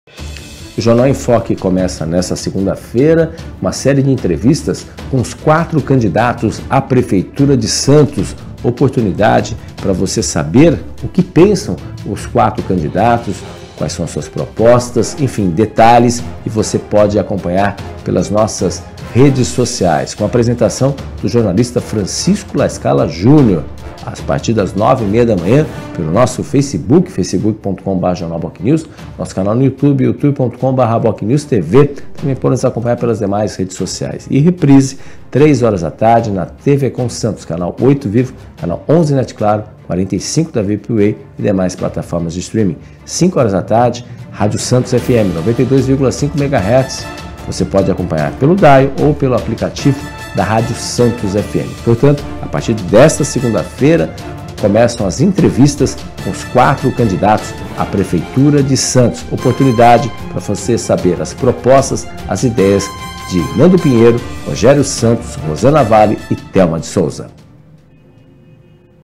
Eleição2024 Confira as entrevistas com os candidatos à Prefeitura de Santos
A partir desta segunda (9), o Jornal Enfoque entrevista os quatro candidatos à Prefeitura de Santos. Apresentação do jornalista Francisco La Scala Jr